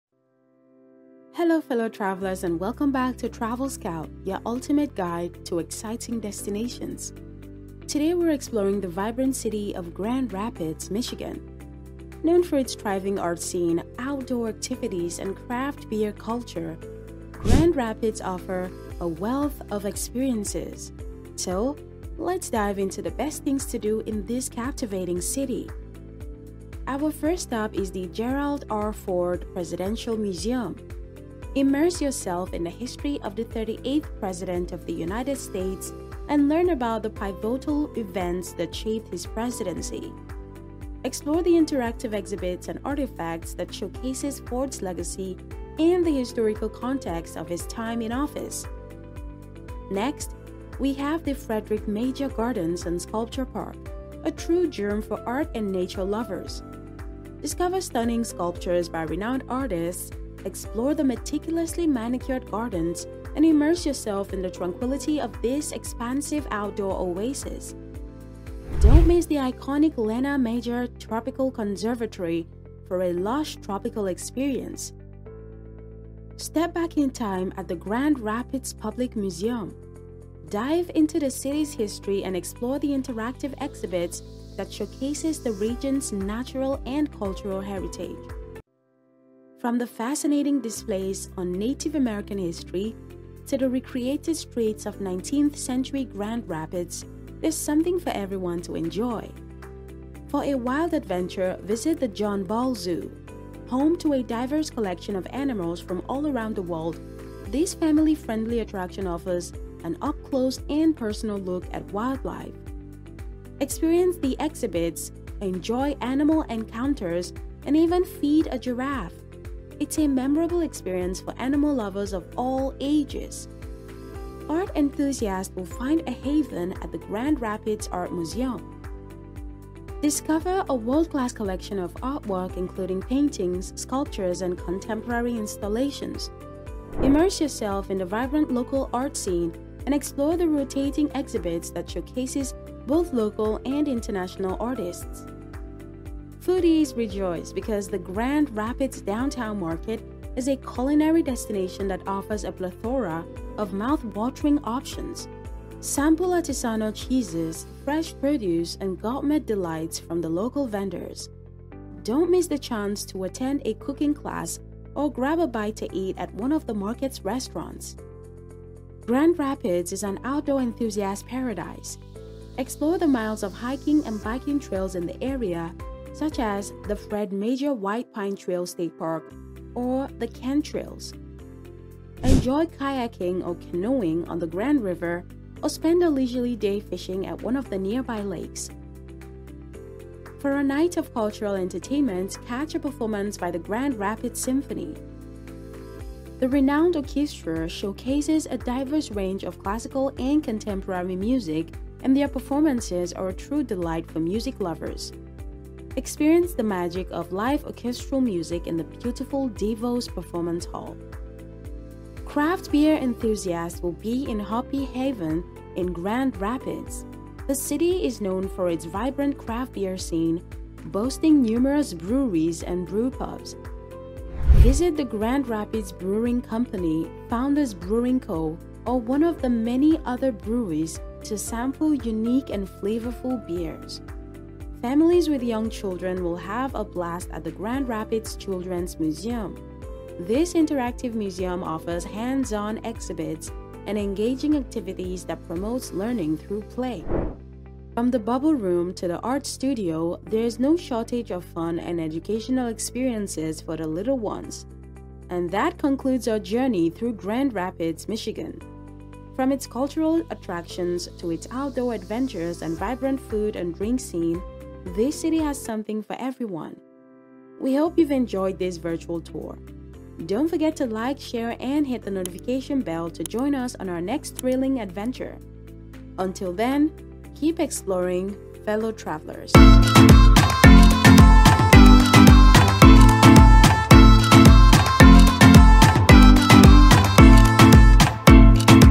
Best Things To Do in Grand Rapids Michigan
Discover the Best Things to Do in Grand Rapids, Michigan | Unveiling Hidden Gems and Cultural Delights! 🌆🎨

Embark on an exciting journey through the vibrant city of Grand Rapids, Michigan, as we unveil the ultimate guide to its top attractions, hidden gems, and cultural delights. From artistic wonders to outdoor adventures, this video showcases the best experiences that Grand Rapids has to offer. Join us as we explore world-class museums, indulge in diverse cuisine, and immerse ourselves in the rich arts and cultural scene of this remarkable city. Don't miss out on this extraordinary adventure that will make your visit to Grand Rapids truly unforgettable! ✨🏛️

🎨 Immerse yourself in the vibrant arts and cultural scene
🍽️ Indulge in diverse and delectable cuisine
🏞️ Discover beautiful parks and outdoor activities
🏛️ Visit renowned museums and historical landmarks
🛍️ Shop at unique boutiques and local markets

🎥 Get ready for a visual feast as we unveil the very best of Grand Rapids, Michigan. 📺🌟